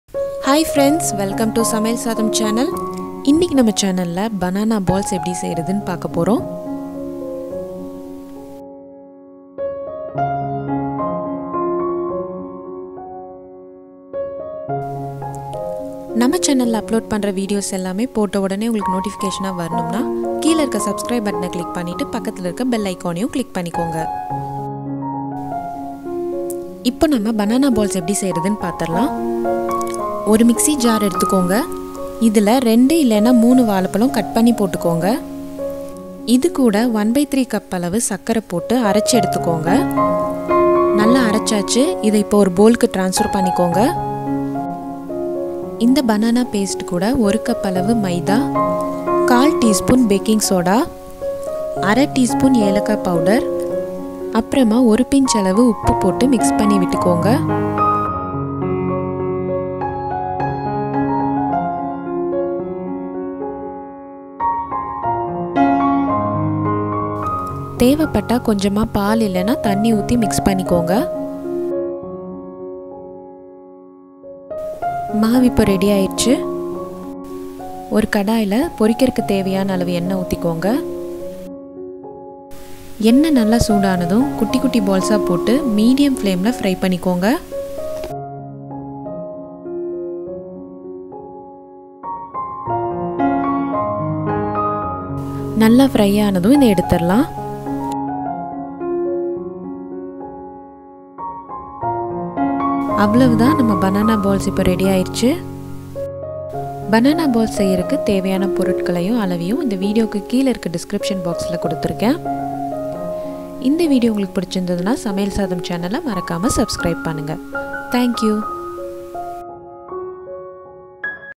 Banana Balls Recipe in Tamil with English Subtitles/Kids Recipe/Snacks/Banana Bonda/SamayalSaadham
Hi Friends,

Welcome to Samayal Saadham Channel.

This video is about how to make Banana Balls Recipe in Tamil with English Subtitles/Kids Recipe/Snacks/Banana Bonda

Ingredients :

2 or 3 bananas,
1/3 cup sugar,
1 cup maida,
1/4 tsp baking soda,
1/2 tsp cardamom powder,
a pinch of salt,
2 tbsp water or milk(if required),

For more details kindly watch the video.